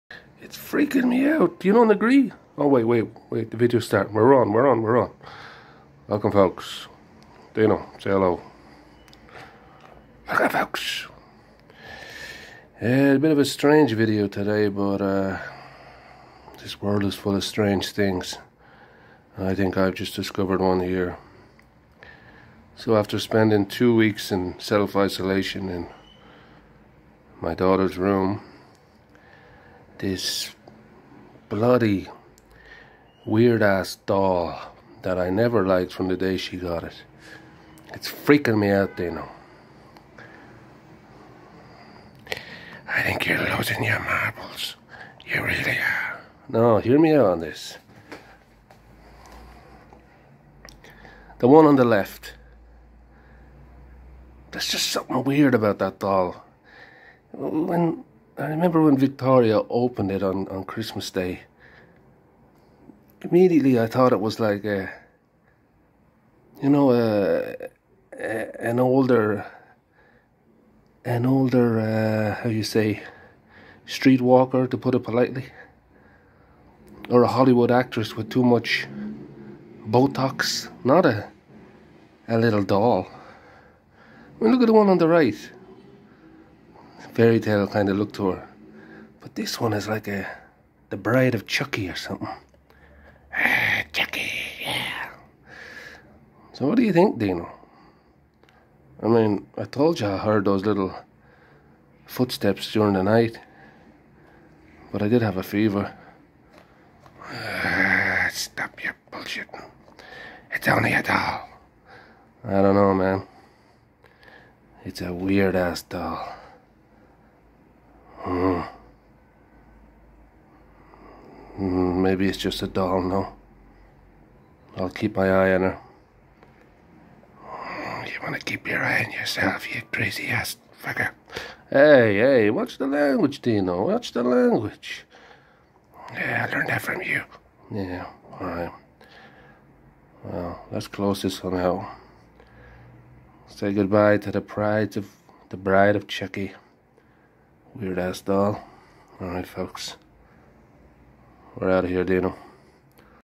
''My Daughter´s Weird-Ass Doll''
Dino is having none of it as I try to explain  my daughter-s weird-ass doll to him.